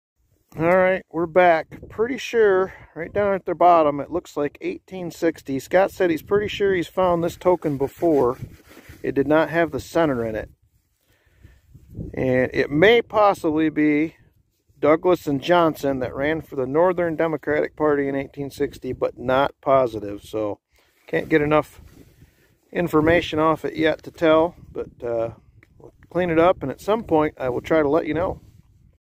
Awesome piece of U.S. History!
We found this on a hunt about a month ago! 1860 Hannibal Hamlin Campaign token. he served as Abraham Lincoln's vice president. what a awesome piece of history